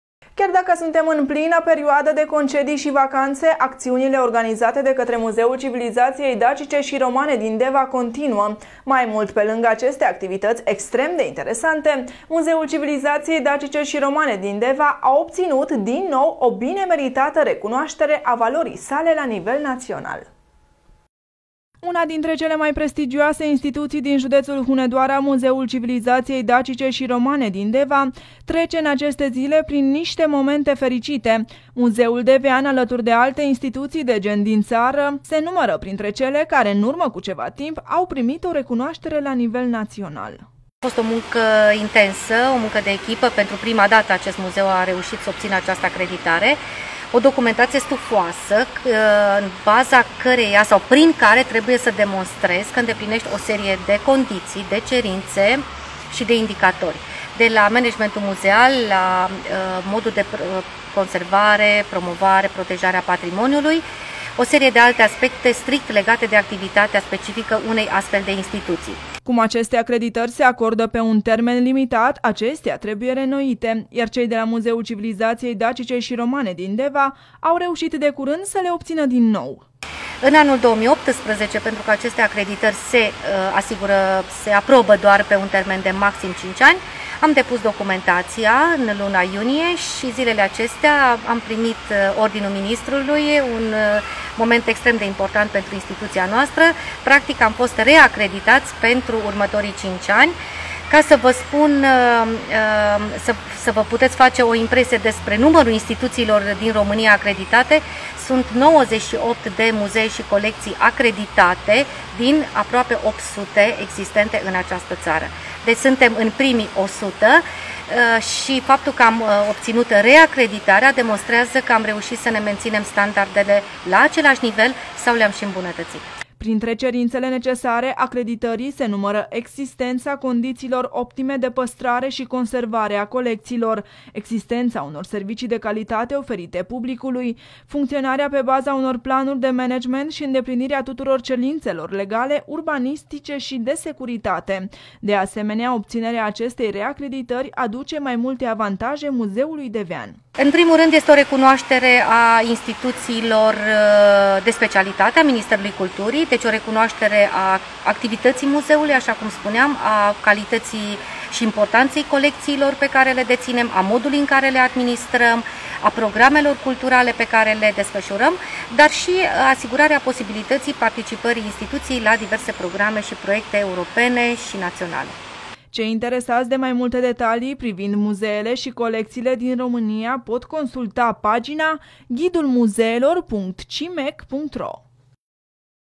Muzeul din Deva a fost reacreditat, 07 2018
Muzeul Civilizaţiei Dacice şi Romane Deva, instituție care funcționează în subordinea Consiliului Județean Hunedoara, anunță pe această cale faptul că în această perioadă a obținut reacreditarea din partea Ministerului Culturii și Identității Naționale. Astfel, în conformitate cu Criteriile și normele de acreditare a muzeelor și colecțiilor publice, reacreditarea instituției noastre s-a făcut prin Ordinul Ministrului nr. 2560 din 13 iulie 2018, în baza Hotărârii Comisiei Naționale a Muzeelor și Colecțiilor nr. 3854 din 26 iunie 2018.

Precizăm faptul că la nivel național există la ora actuală un număr de 96 de instituții muzeale sau colecții publice acreditate, dintr-un total de aproximativ 800. Cei interesați de mai multe detalii privind muzeele și colecțiile din România pot consulta pagina ghidulmuzeelor.cimec.ro.

Printre cerințele necesare acreditării se numără existența condițiilor optime de păstrare și conservare a colecțiilor, existența unor de servicii de calitate oferite publicului, funcționarea pe baza unor planuri de management și îndeplinirea tuturor cerințelor legale, urbanistice și de securitate.